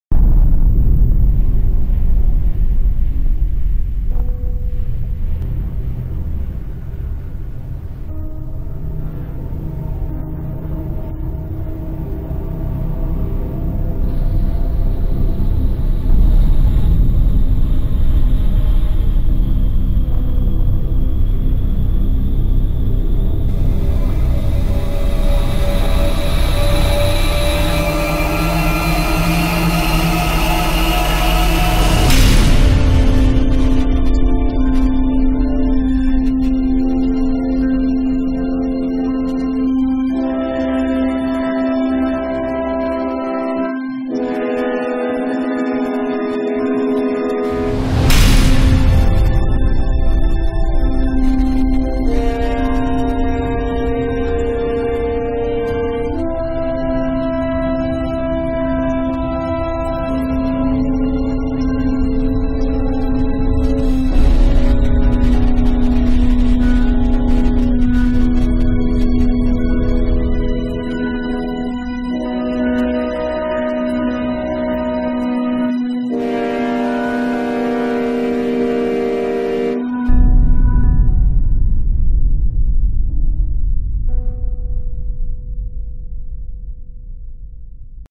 Friday the 13th Part VIII: Jason Takes Manhattan Replica Mask
Closer look image gallery video for the Friday the 13th Part VIII: Jason Takes Manhattan replica mask by Jason Creation Station. o

Music written by Tom Cusack